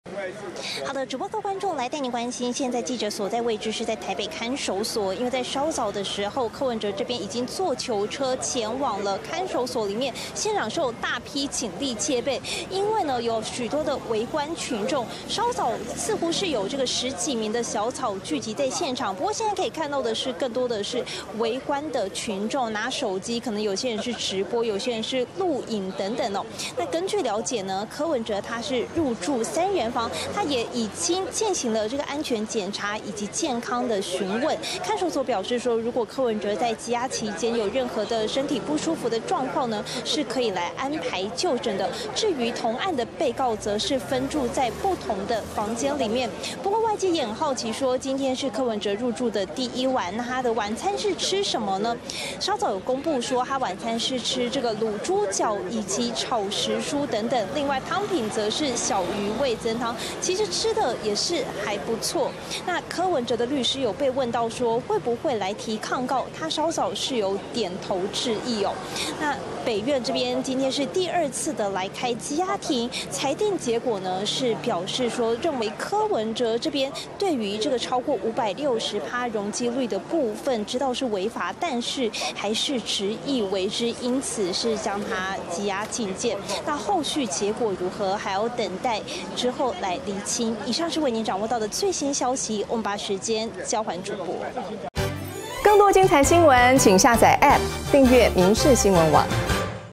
柯文哲上銬送北所住3人房！沒冷氣熱水 今早吃「銀絲卷+奶茶」三餐菜單曝光－民視新聞
【民視即時新聞】社會中心／黃恩琳、戴亞倫　台北報導前台北市長柯文哲捲入京華城案，週四（5）下午2度開羈押庭，最後遭羈押禁見，搭載柯文哲的囚車，也在晚間6點33分抵達台北看守所，許多柯黑、小草聚集在外頭呼喊。台北看守所第一晚的菜色也曝光了，有滷豬腳、紅燒魚丸等等，有網友說「吃得比我好」，不過柯文哲因是晚間6時許才抵達看守所，所以並沒有吃到晚餐。